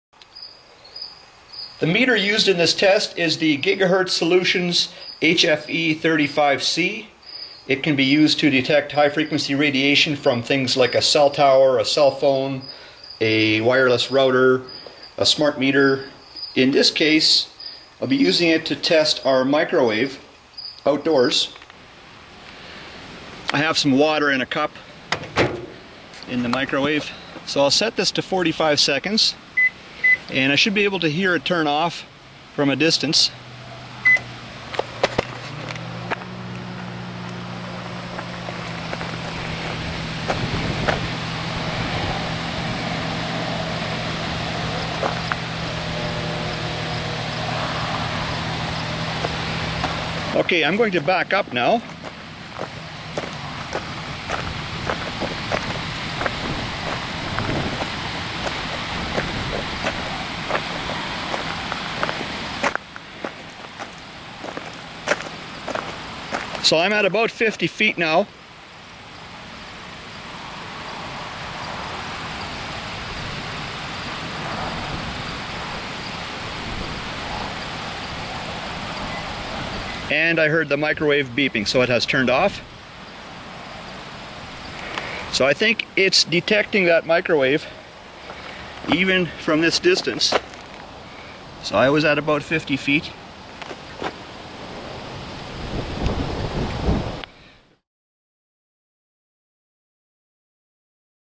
Out Door Microwave test
Showing results from about 50 feet distance.